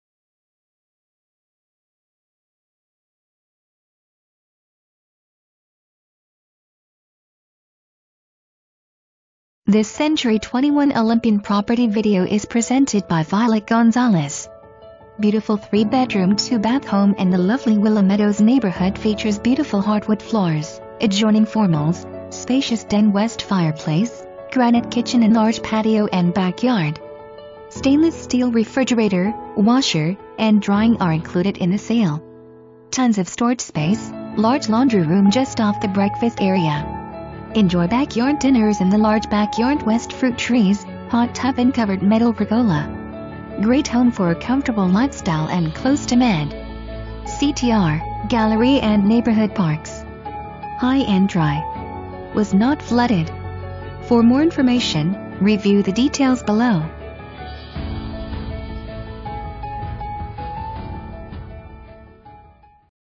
Century 21 Olympian - FOR RENT 4422 NENANA DRIVE HOUSTON, TX 77035
Please enjoy this property video created by Century 21 Olympian

FOR RENT 4422 NENANA DRIVE

Beautiful 3-bedroom 2 bath home in the lovely Willow Meadows neighborhood features beautiful hardwood floors, adjoining formals, spacious den w/fireplace, granite kitchen & large patio & back yard! Stainless steel refrigerator, washer, and drying are included in the sale. Tons of storage space, large laundry rm just off the breakfast area. Enjoy back yard dinners in the large back yard w/fruit trees, hot tub & covered metal pergola. Great home for a comfortable lifestyle & close to Med. Ctr., Galleria & neighborhood parks. High and Dry. Was not flooded.

Violet Gonzalez
Century 21 Olympian